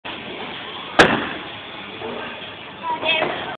Daniel from 1a2 hillgrove 2008 'longkang' king
he kept shooting in the drain . wads wrong with him ?